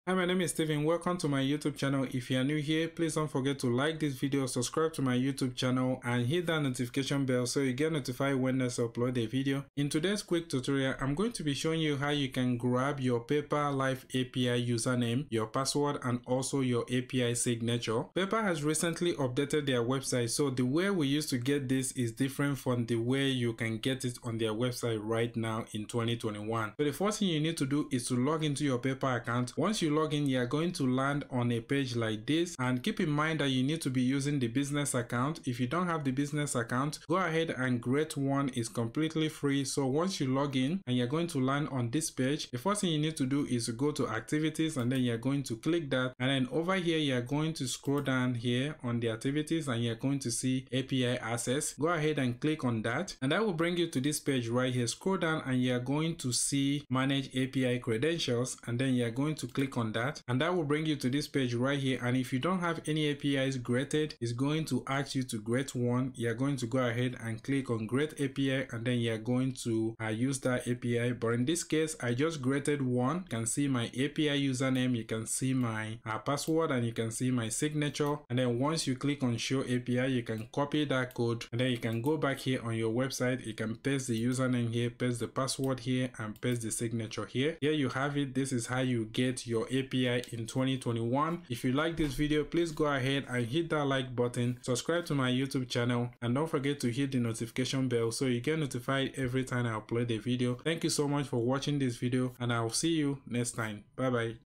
How to Get PayPal API Credentials | 2021
In this short video, I will show you how you can get PayPal API credentials.

☑ Watched the video!
☐ Liked?